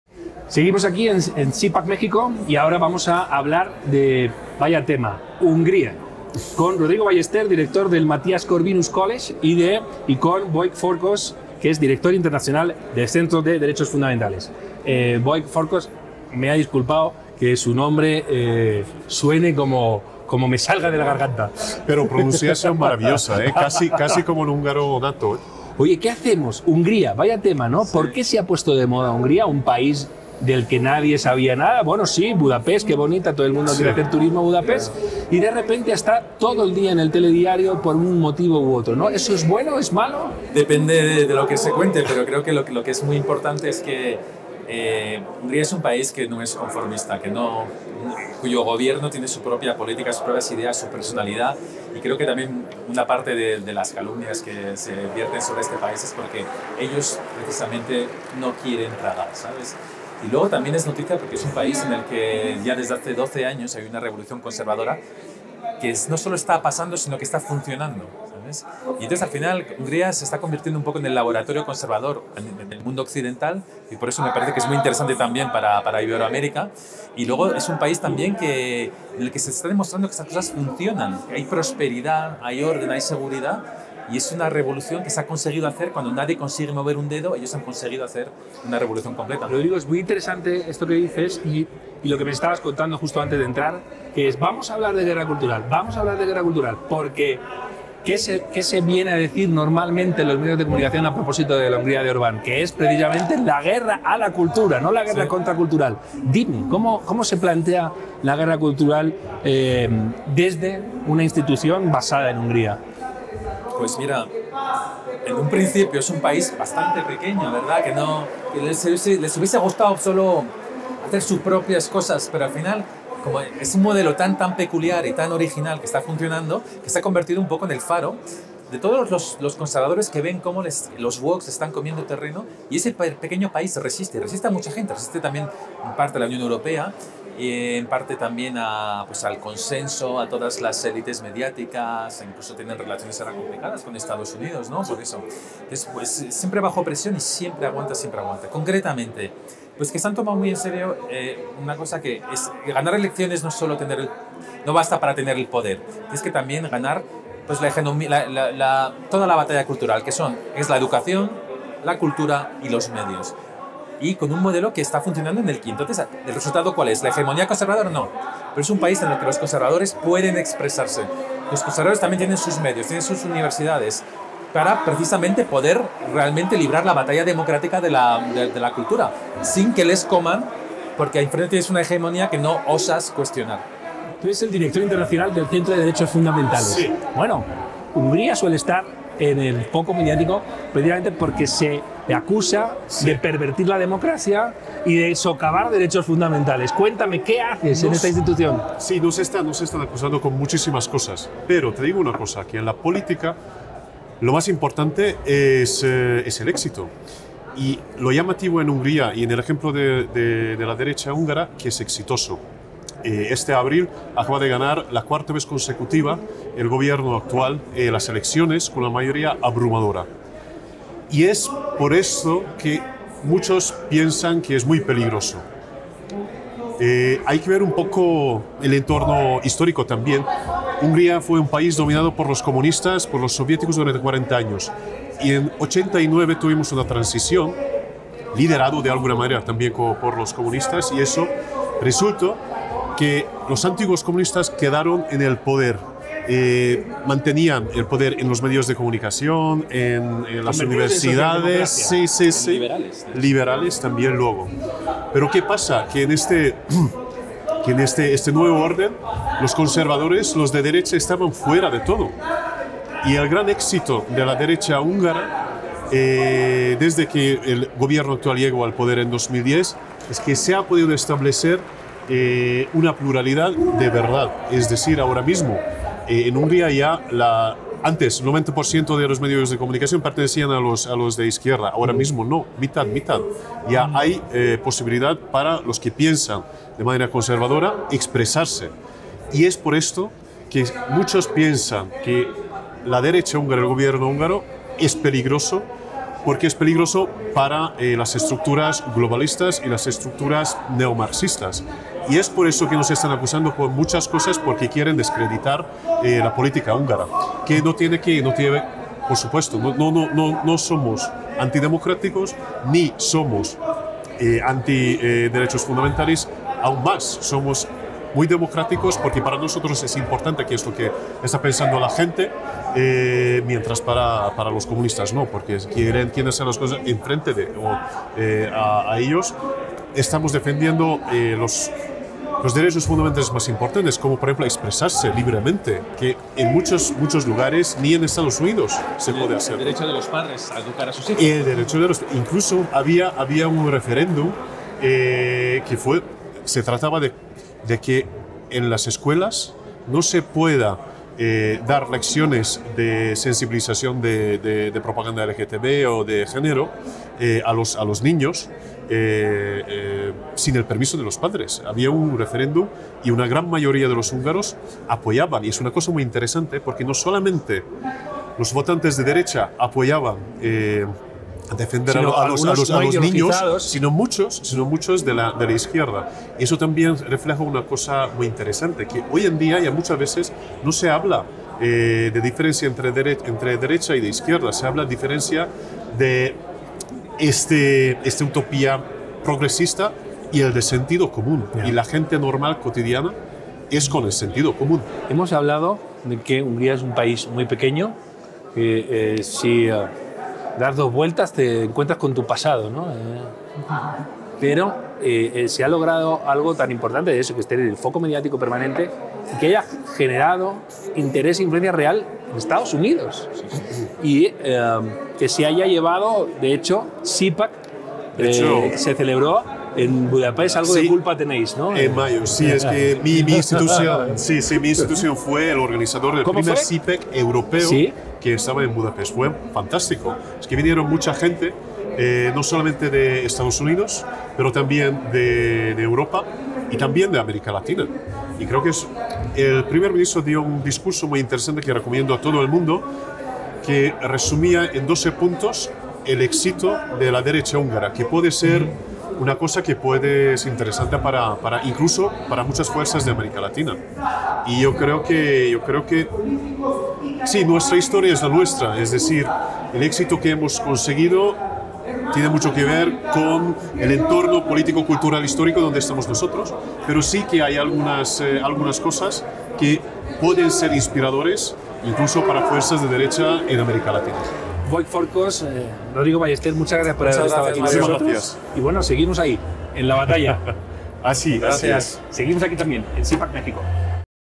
CPAC México 2022: Mario Noya conversa con Rodrigo Ballester y Vajk Farkas
El director de voz.us conversa sobre Hungría con Rodrigo Ballester, director del Centro de Estudios Europeos del Mathias Corvinus Collegium de Budapest, y Vajk Farkas, director de Relaciones Internacionales del Centro para los Derechos Fundamentales de Budapest.